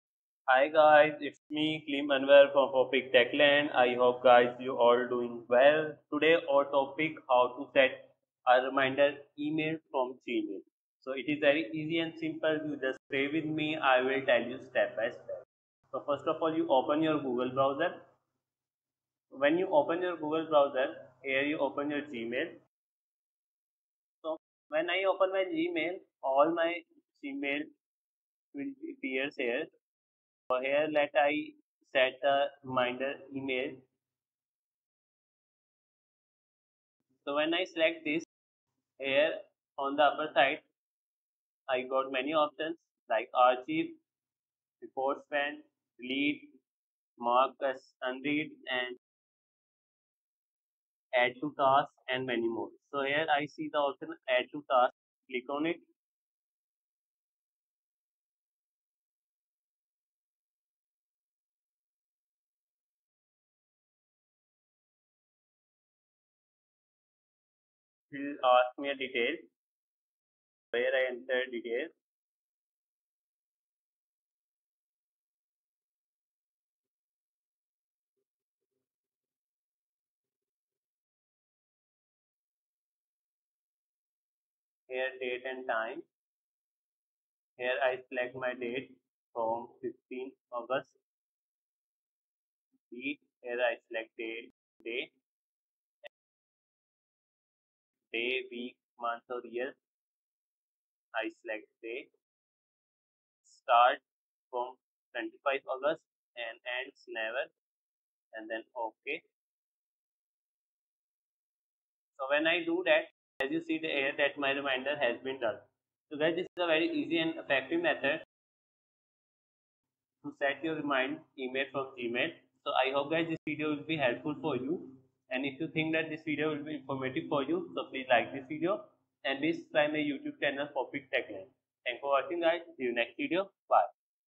How to Send a Reminder Email from Gmail (SOLVED)
How to Send a Reminder Email from Gmail

In this video, i will show you How to Send a Reminder Email from Gmail. This is very easy method. I hope it will help you. You can Send a Reminder Email from Gmail by following the steps.